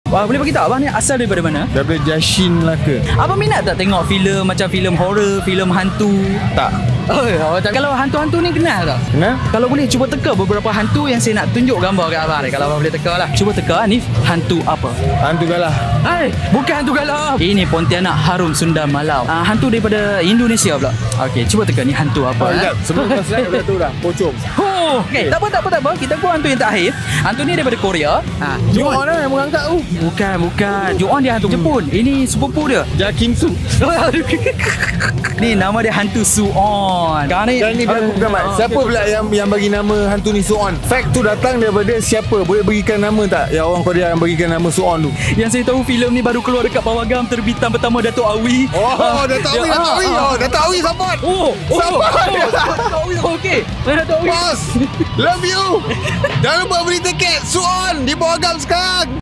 BIDIN AL ZAIFA MARAH SIAPA BAGI NAMA HANTU SUE-ON
@bidinalzaifa dah ajak tengok Filem Sue-On tu. Jom ramai-ramai tengok Sue-On di pawagam berdekatan anda sekarang!

🔥Follow akaun rasmi filemsueon untuk update terkini🔥